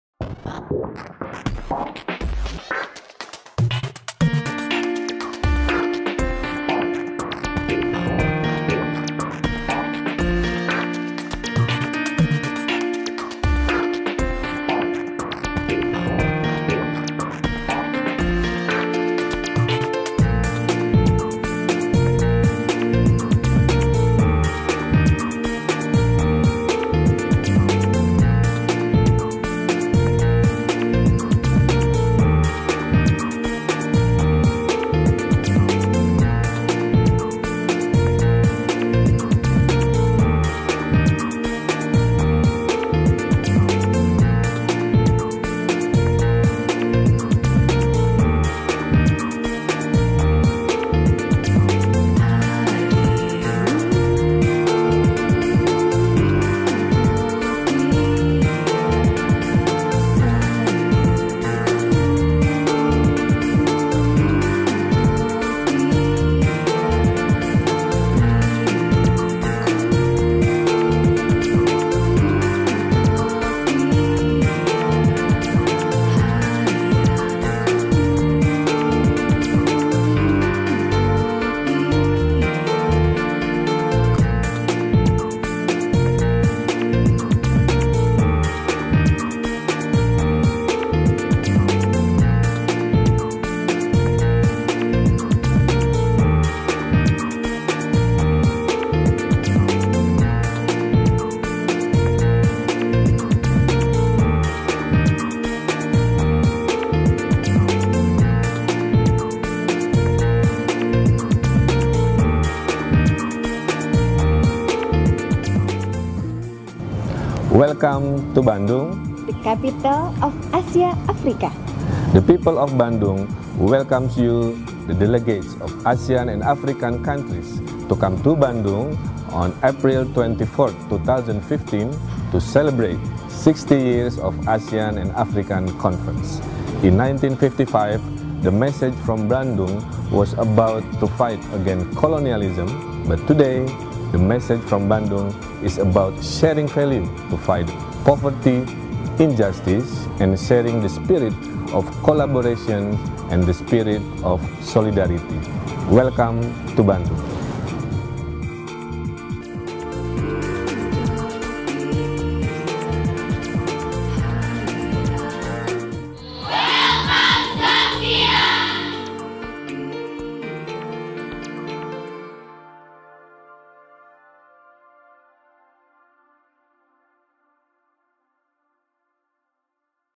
From Bandung to The People of Zambia
Welcome to Bandung, the city made of happiness. The people of Bandung welcomes you, the delegates of Asian and African countries, to come to Bandung on April 24 2015 to celebrate 60 years of Asian and African Conference. In 1955, message from Bandung was about to fight against the colonialism. Today, the message from Bandung is about sharing value to fight poverty, injustice, and sharing the spirit of collaboration and the spirit of solidarity. Welcome to Bandung.